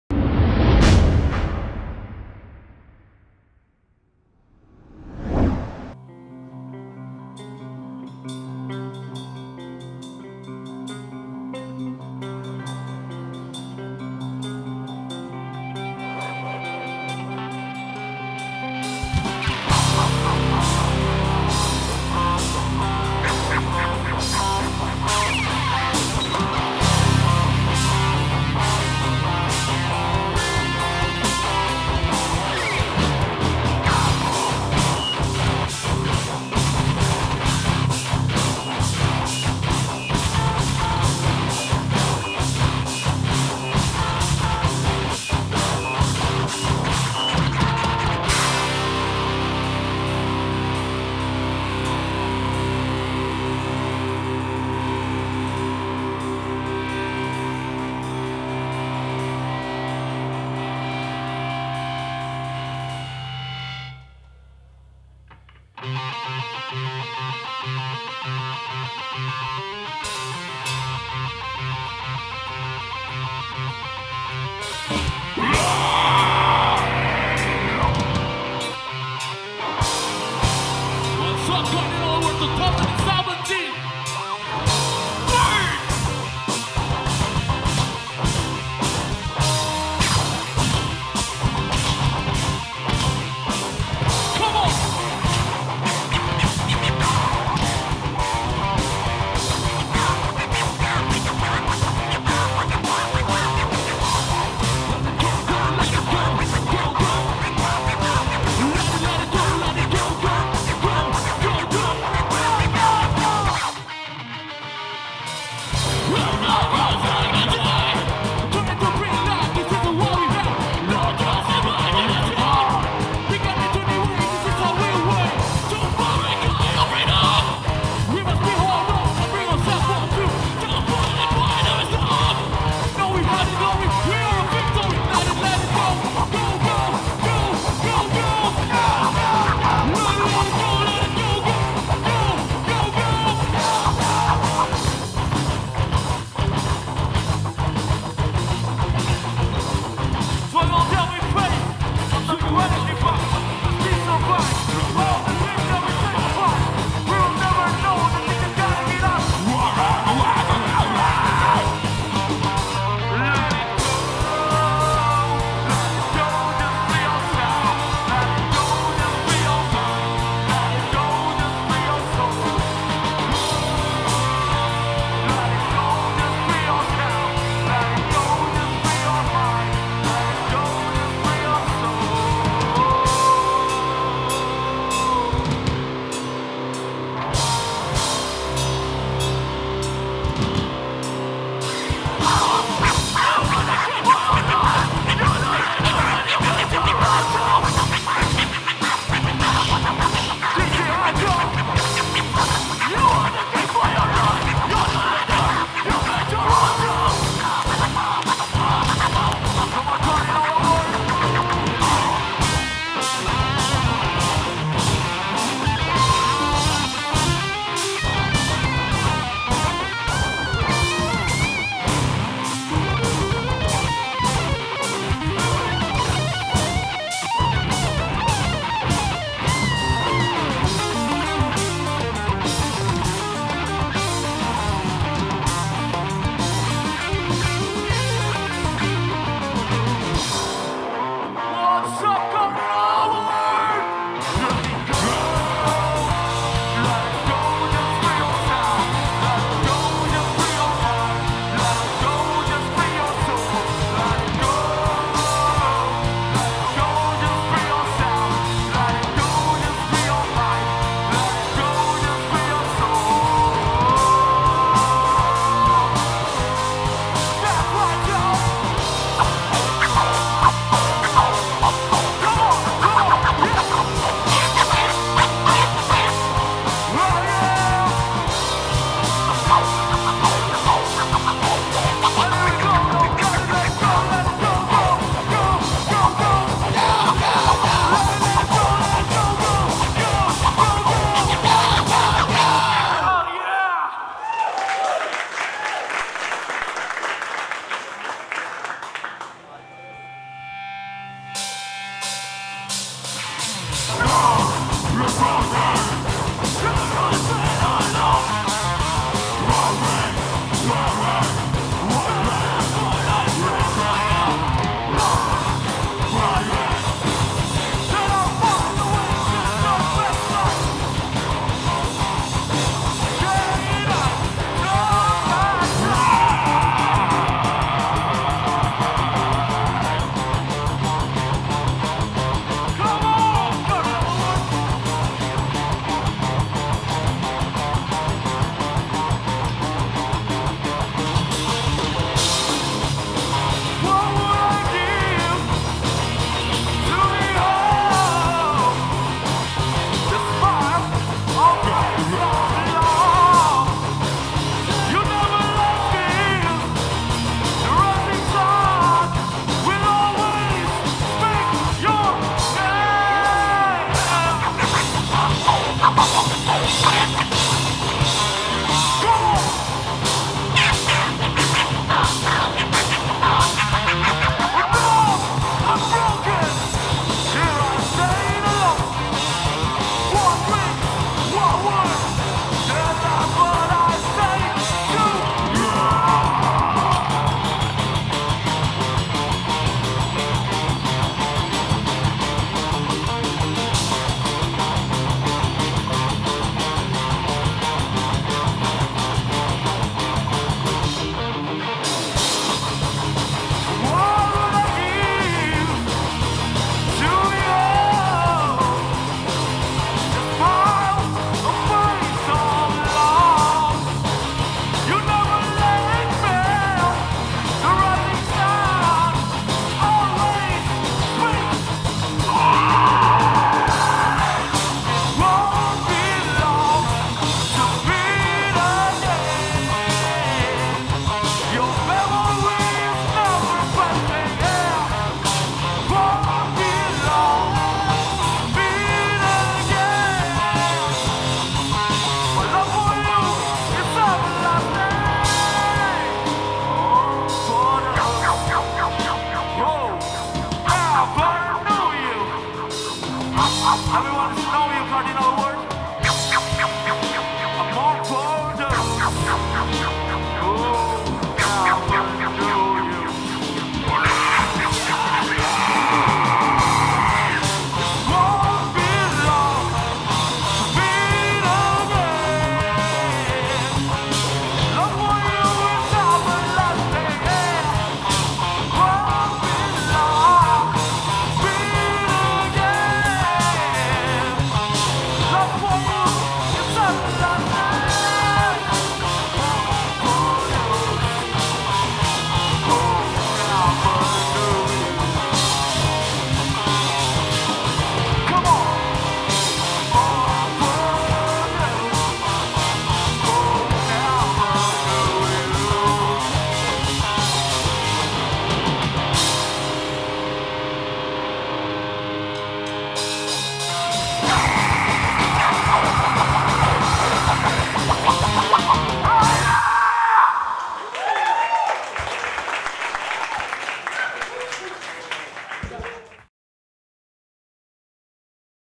CARDINAL AWARDS 2017
MEDAN Best Band, from CARDINAL AWARDS 2017 Audition @ ISMUD Park, August 1st : JAGER MONSTER